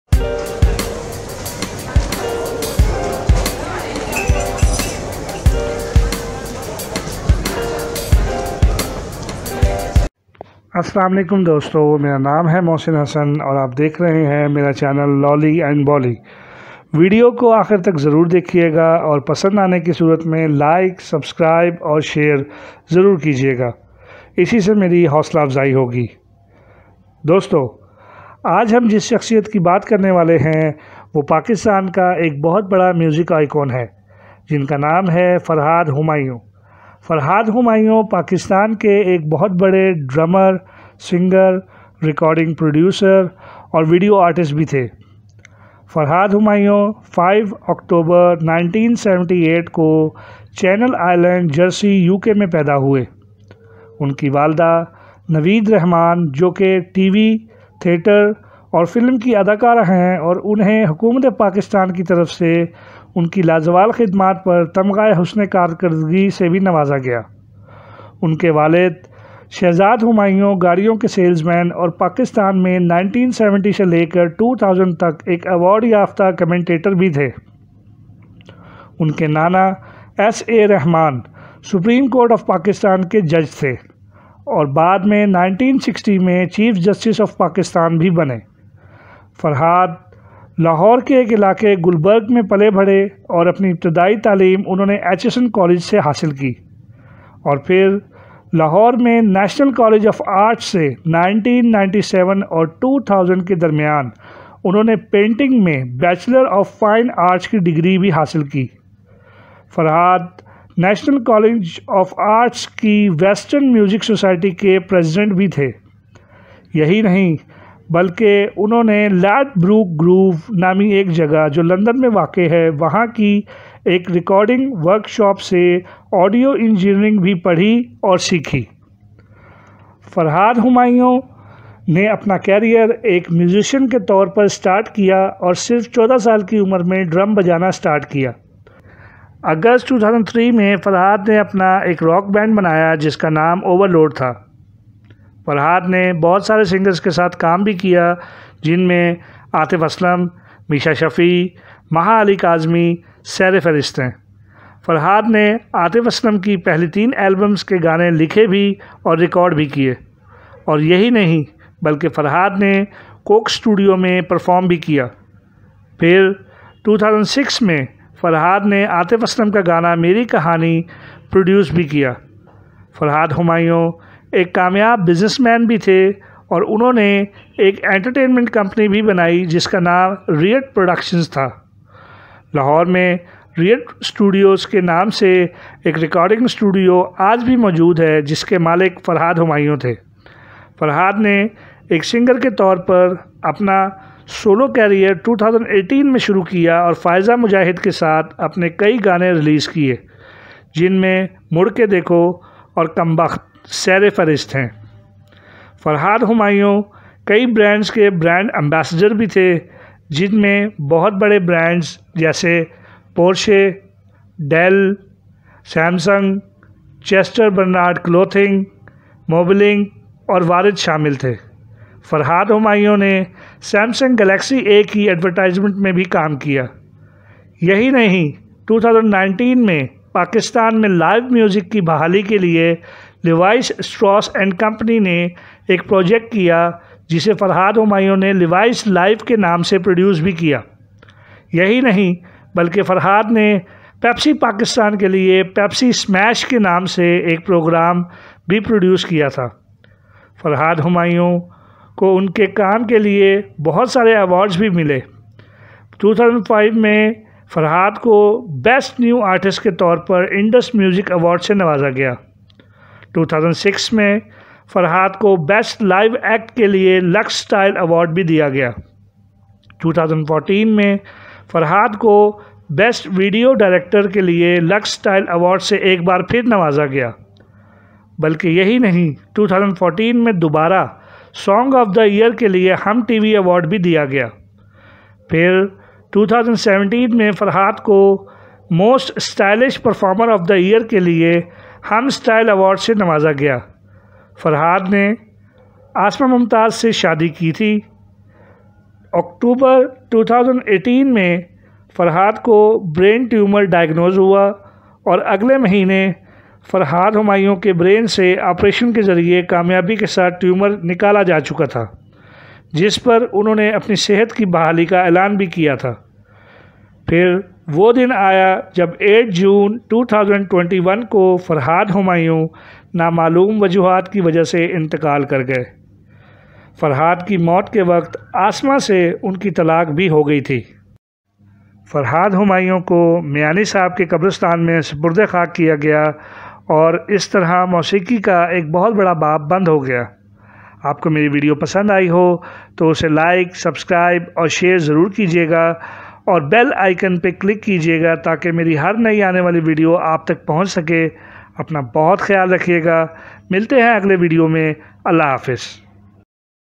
Farhad Humayun Biography & Info.
Assallam o Allaikum !
In this video I'm gonna tell you about the Lollywood Celebrity Date of Birth, Place of Birth, Childhood Life, Career, Achievements, Facts and their all Personal Information & much more. . .